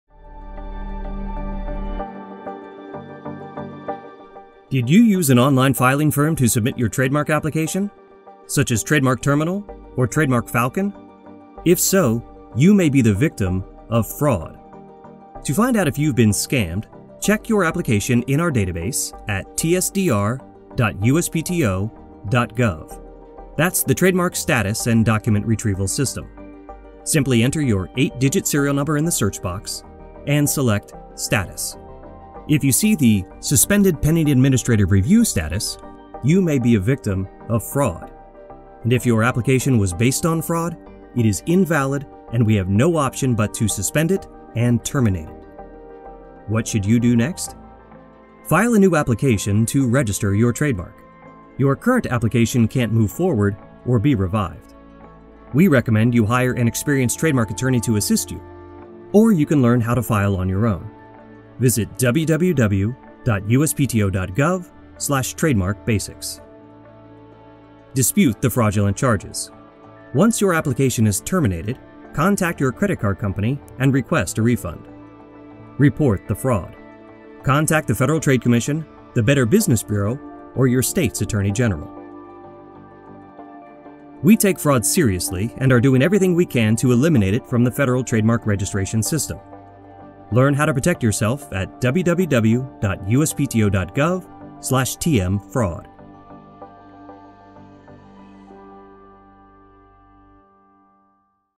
Protect yourself against trademark fraud
Did you use an online filing firm to submit your trademark application? If so, you may be the victim of fraud. To find out if you’ve been scammed, and learn what to do next, watch this short video.

We take fraud seriously and are doing everything we can to eliminate it from the federal trademark registration system.